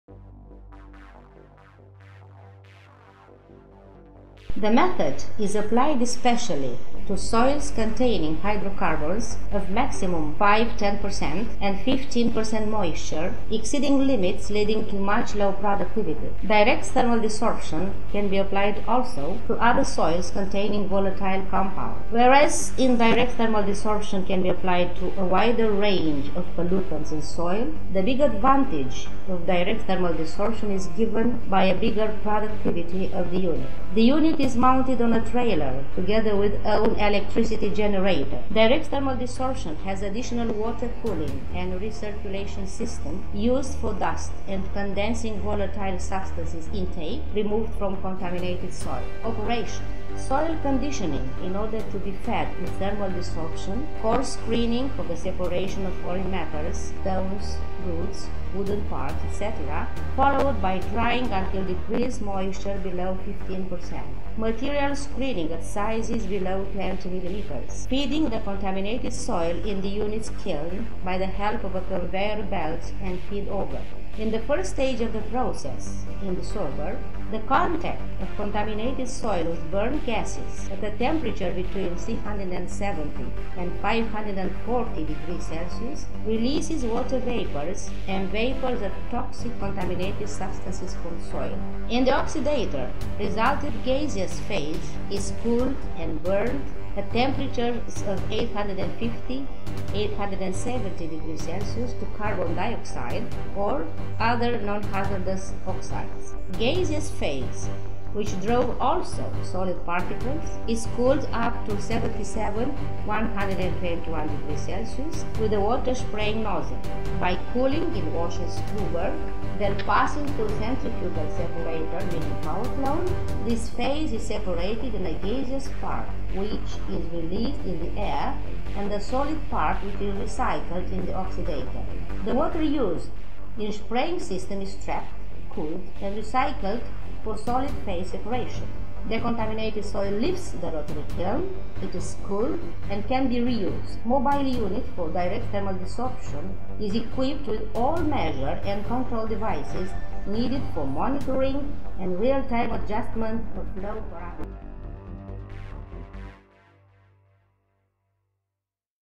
MOBILE UNIT FOR DIRECT THERMAL DESORPTION
Whereas indirect thermal desorption (IDT) can be applied to a large range of pollutants in soil, direct thermal desorption (DTD) is especially used for the treatment of soils contaminated with hydrocarbons and/or other pollutants containing volatile compounds.
Hydrocarbons concentration (TPH) in soil should not exceed 10% and moisture 15%, exceeding such limits could lead to considerably low productivity.

Operation, stages, results:

    soil conditioning in order to be introduced in thermal desorption plant – coarse screening for foreign matters separation (stones, roots, wooden pieces etc.), followed by drying until moisture decreases below 15%;
    introducing contaminated soil in the desorber, on a conveyor belt after a fine screening up to a granulation of max. 3 mm;
    in the first stage of the process, in desorber, contaminated soil comes directly into contact with combustion gases, at controlled temperature between 370 and 540oC, water vapors and vapors of contaminated toxic substances from soil being released;
    in the second stage, in oxidator, resulted gaseous phase is heated and oxidized at temperatures of 850 – 870oC, until getting carbon dioxide or other non-hazardous oxides;
    in the third stage, gaseous phase, which drove also solid particles, is cooled up to abt. 77 – 120oC by the help of a water spraying system; by cooling in washing scrubber and then passing through the powercyclone, this phase is separated into a non-hazardous gaseous component which is released in the air and a solid component which is recycled in the oxidator;
    the water used in spraying system is entrapped, cooled and recycled in order to retain the dust and condesable volatile substances removed from contaminated soil;
    the soil decontaminated in the first stage is cooled, checked by specific laboratory analyses and stored for being reused.

The major advantage of direct thermal desorption is a bigger productivity compared with indirect thermal desorption. This advantage is due to both the fact that the soil contaminated comes into direct contact with the combustion gases in the desorber, as well as the fact that the petroleum products from soil maintain combustion, improving the unit efficiency.

The mobile unit, including also its electricity generator, is mounted on a trailer, which enables easy assembly in areas close to contaminated site and implicitly a considerable reduction of costs for contaminated soil transport.
Mobile unit for indirect thermal desorption is equipped with all measure and control devices needed for monitoring and adjusting in real time of flow parameters.